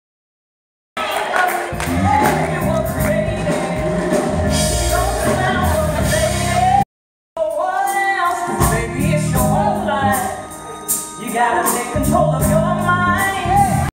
Something to Talk About Scene 9 stage play by Kizzy Fore
Sharon confronts Alicia in song
Something to Talk About Stage play by Kizzy Fore
featuring T'Marah Esi and Raven Jones